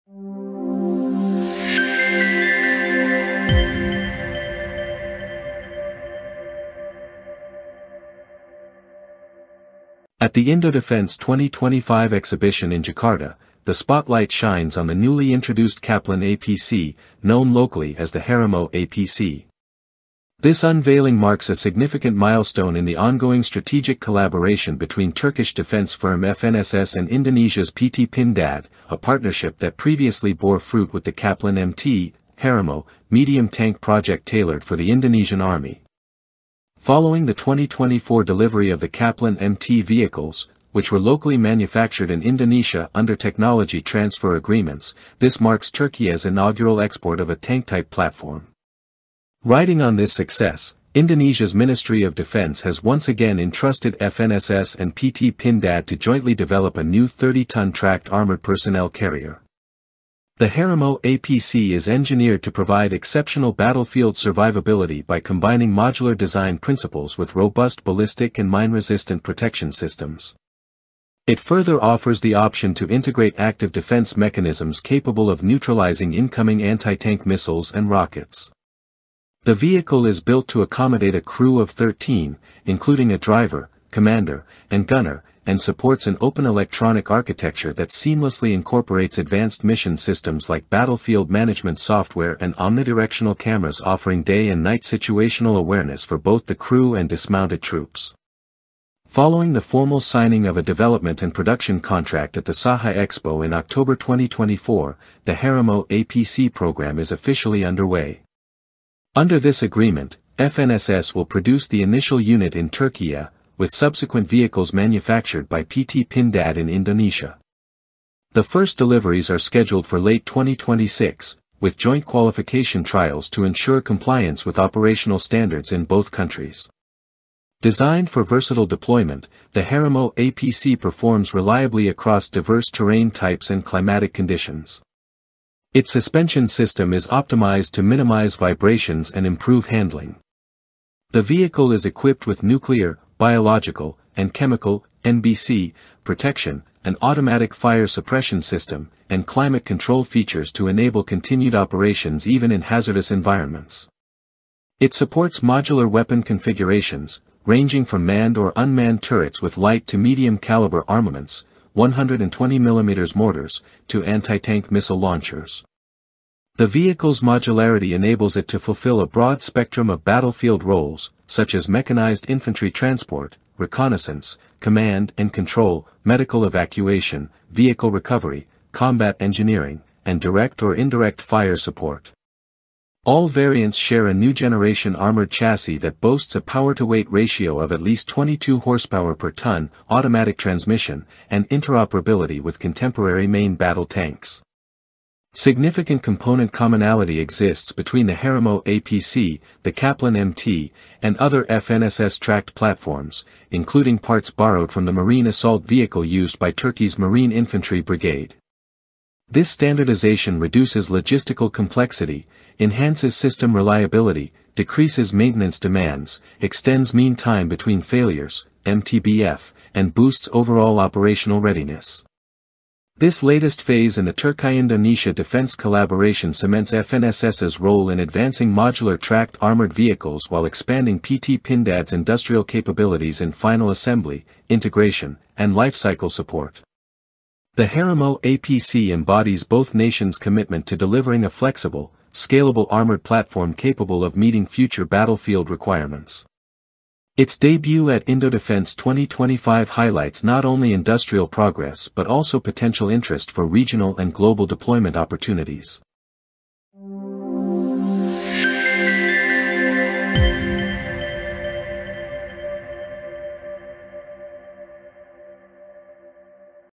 Indonesia and Türkiye Unveil Advanced HARIMAU APC at IndoDefence 2025
The HARIMAU APC, unveiled at IndoDefence 2025, is a modular 30-ton tracked armored personnel carrier developed by FNSS and PT Pindad. Designed for high survivability, advanced protection, and multi-role capabilities, it strengthens Türkiye-Indonesia defense collaboration and regional security.